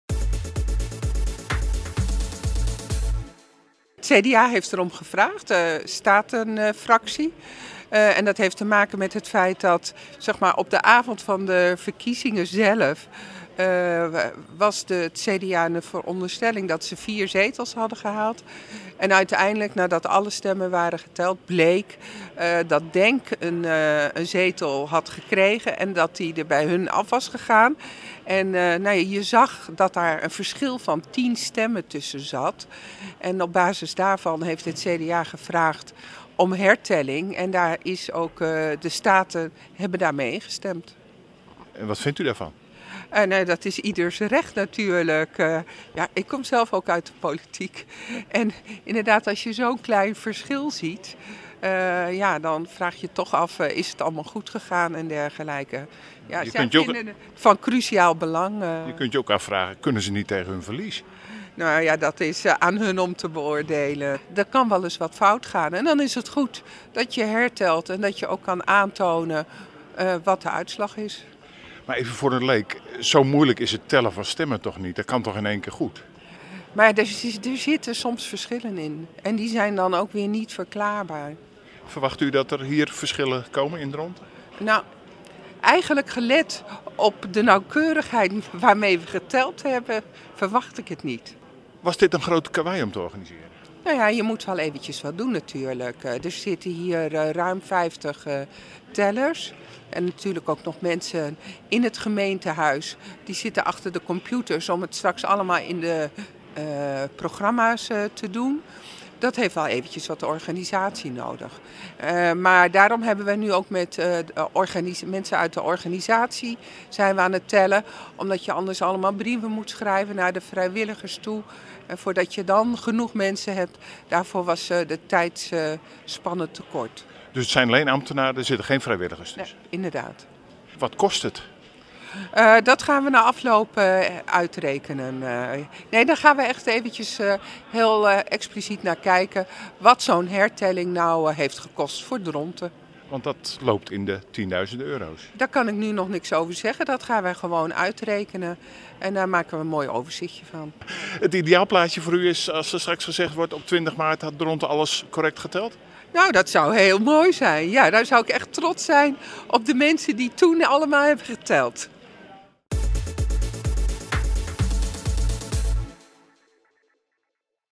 Ineke Bakker over hertelling Statenverkiezingen in De Meerpaal in Dronten
DRONTEN - Op verzoek van de CDA-statenfractie worden alle stemmen van de verkiezingen van 20 maart opnieuw geteld. In de gemeente Dronten gebeurt dat door vijftig ambtenaren op het binnenplein van De Meerpaal. Kan het CDA niet tegen z’n verlies?  „Dat is aan hun om te beoordelen”, reageert burgemeester Ineke Bakker in haar functie als hoofd stembureau, „Er kan wel eens wat fout gaan.”